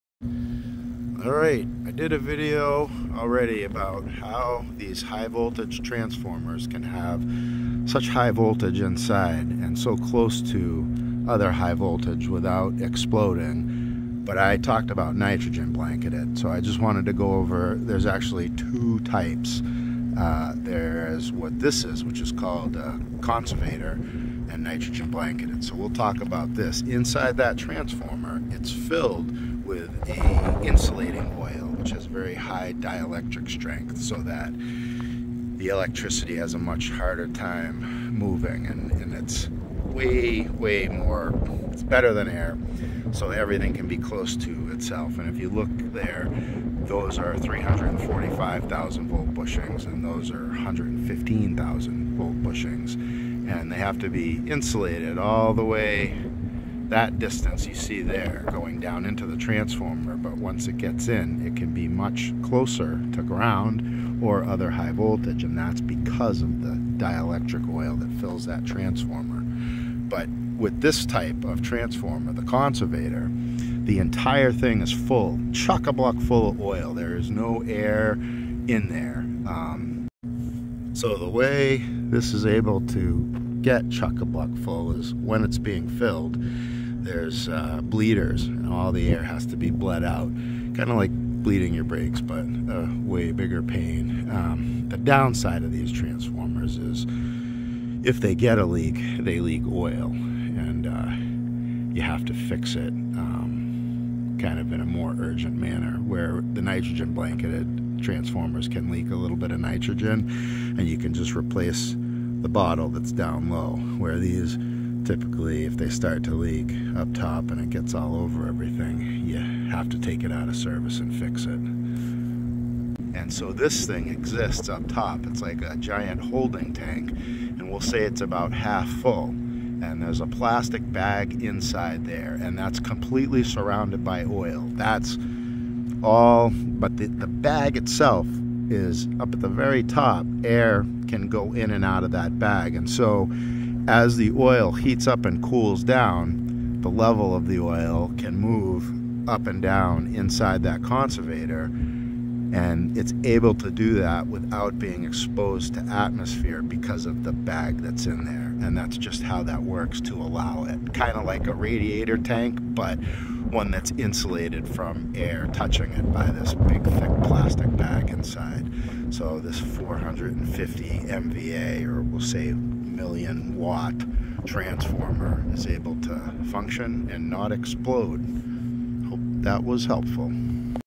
Why transformers don't explode with 345kv inside - conservator style explained
Just a real quick overview on transformer insulating oil and how it is kept from touching atmosphere.
If it is exposed to atmosphere,  it will absorb moisture and lose its insulation value.